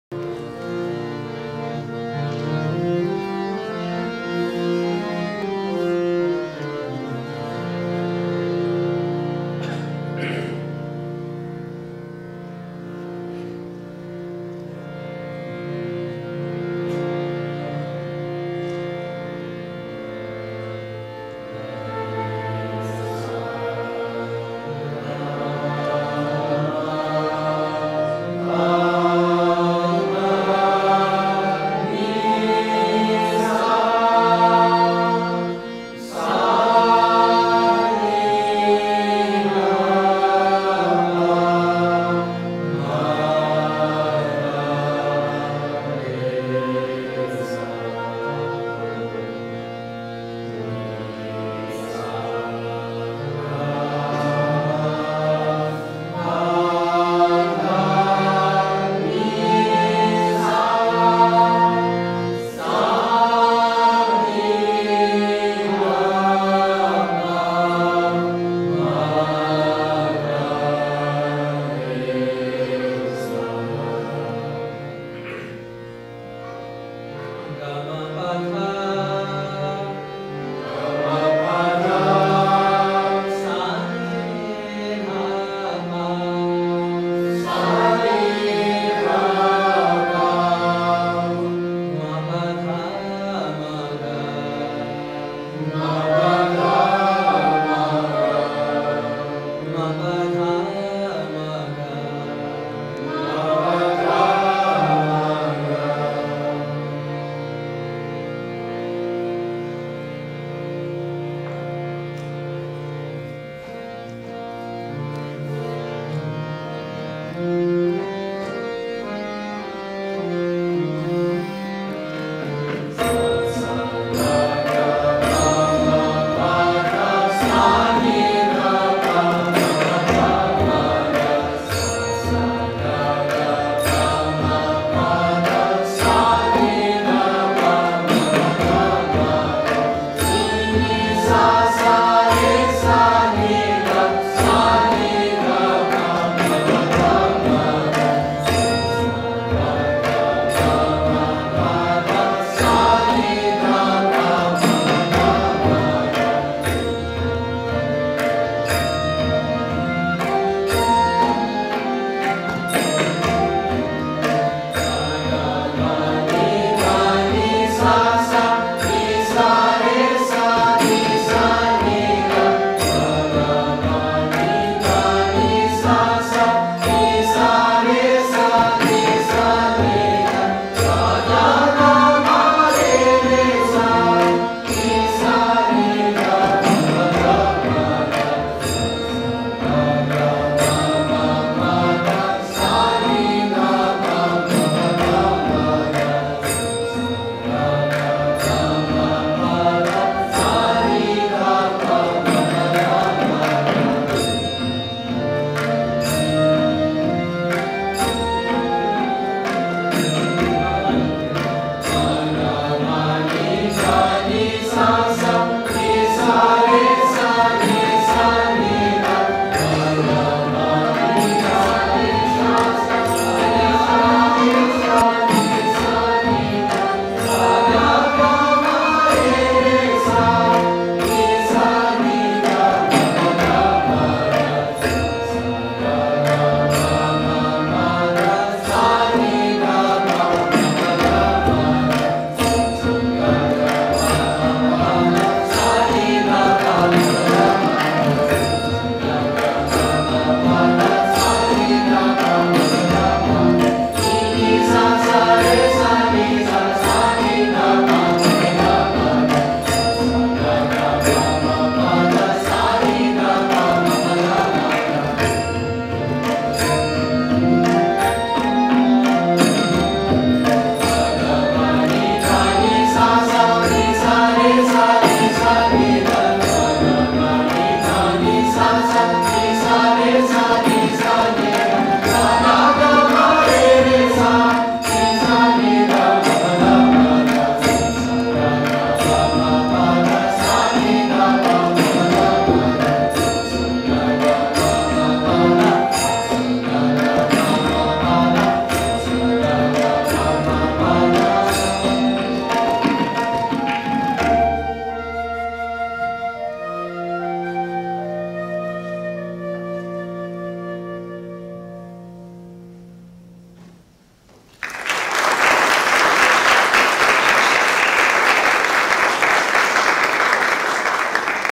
Raag Khamaj Sargamgeet.mp4
Sargamgeet in Raag Khamaj, set to Teentaal. Auburn Indian Music Ensemble, Spring Concert, April 2011. On Flute: Morgan Stephens, on Tabla: Achintya Sujan. Directed by Dr. S. Raj Chaudhury